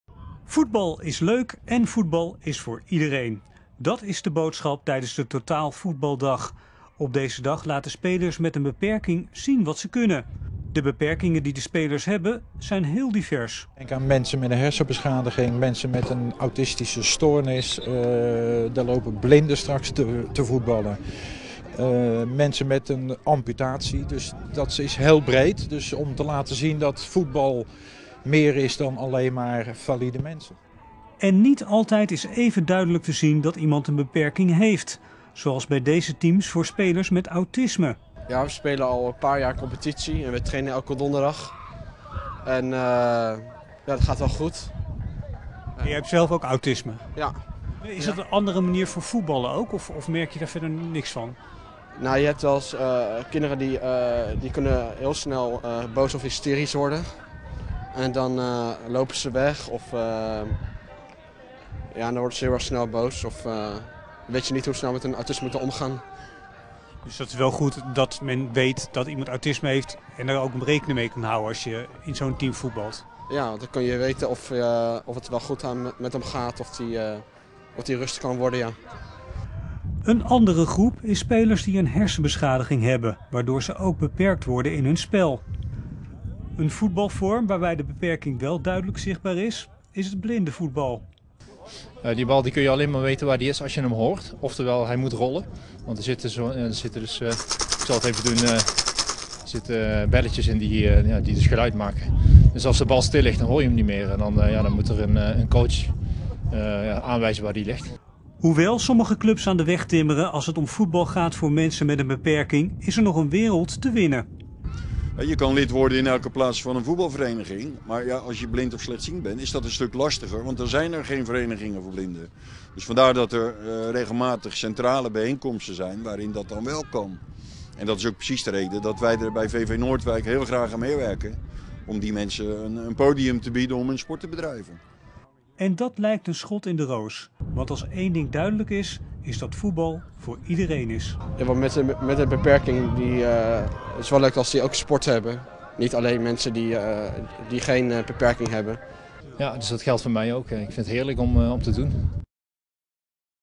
Totaalvoetbal is voor iedereen
Een primeur voor Noordwijk. Bij de voetbalvereniging daar werd vandaag
de eerste editie van de Totaalvoetbaldag gehouden. Het evenement, dat
wordt georganiseerd door de KNVB, VV Noordwijk en de gemeente
Noordwijk, moet meer bekendheid geven aan de aangepaste voetbalvormen
die er zijn voor spelers met een beperking.